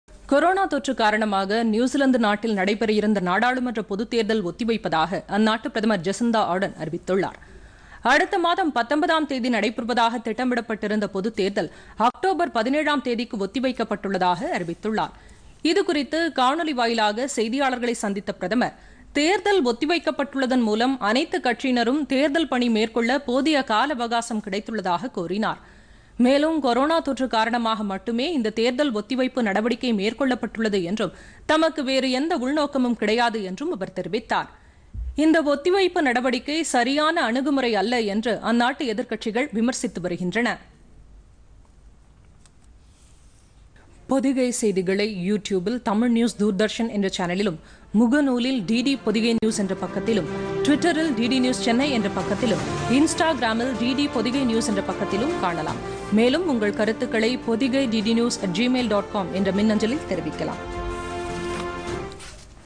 கொரோனா - நியூசிலாந்தில் தேர்தல் ஒத்திவைப்பு
கொரோனா  தொற்று இரண்டாவது முறையாக பரவி வருவதால் நியூசிலாந்தில் பொது தேர்தல் ஒத்தி வைப்பு - பிரதமர் ஜெசிண்டா ஆர்டர்ன் அறிவிப்பு